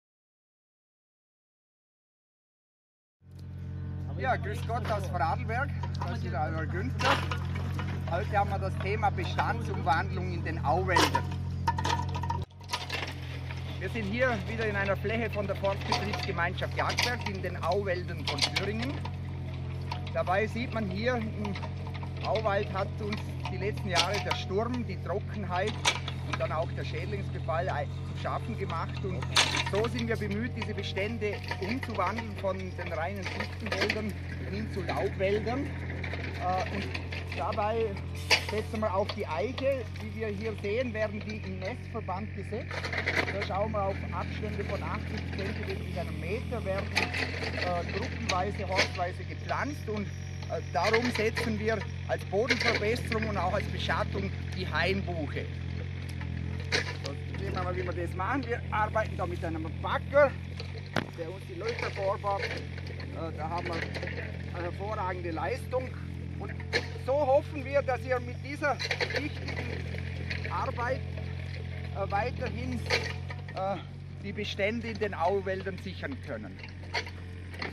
Günter baut seinen Auwald um
Eine neue Video-Geschichte aus dem Wald 🎬:
Günter befindet sich gerade im Auwald, der durch den Klimawandel und seine Folgen wie Sturm, Trockenheit und Schädlingsbefall sehr gelitten hat. Damit dieser klimafit wird, möchte Günter nun den Wald umbauen und setzt auf zusätzliche Baumarten.

Neben der Eiche, die in sogenannten Trupps gesetzt werden, fügt er auch die Hainbuche hinzu. Diese soll die Eichen beschatten, damit diese möglichst astreine Stämme erhalten, zusätzlich verbessert das Laub der Hainbuche den Boden. Mit dieser Maßnahme hofft Günter, dass er die Bestände in den Auwäldern sichern kann.